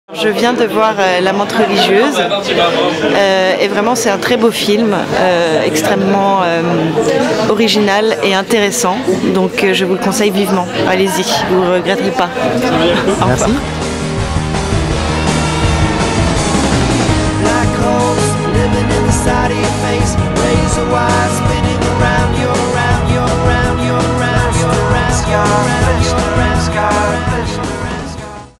Joséphine de Meaux : « Allez le voir, vous ne le regretterez pas ! »
L'avis de l'actrice Joséphine de Meaux après l'avant-première de La Mante Religieuse, le 28 au cinéma Publicis sur les Champs-Elysées.